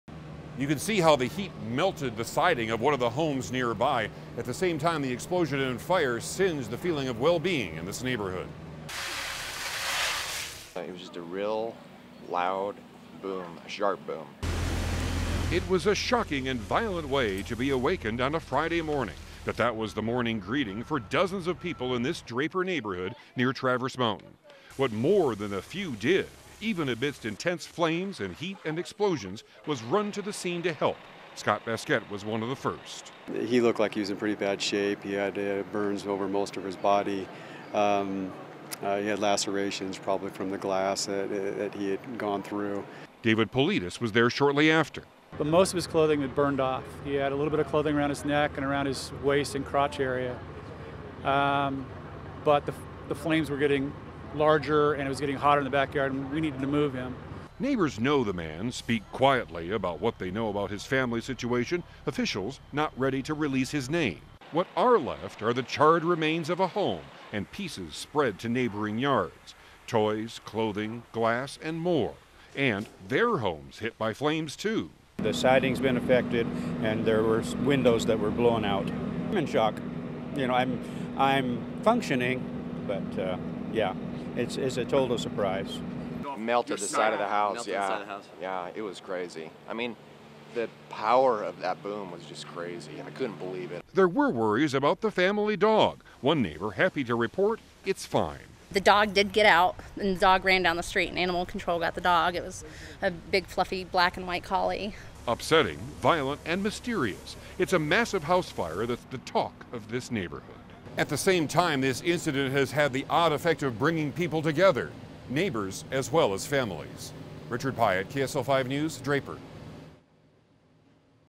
A House Explodes in Draper, UT
The aftermath of an explosion and fire in a neighborhood.